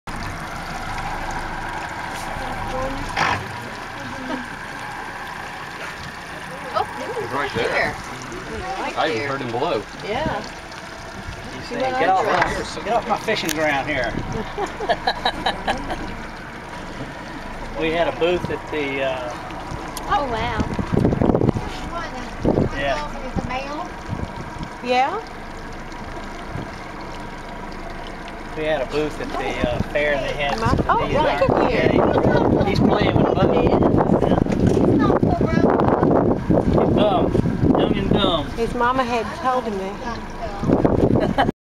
Dolphin playing with boat
A young dolphin plays with our boat on the way to Blackbeard Island, Georgia. You can also see the dolphin "footprints" in the water.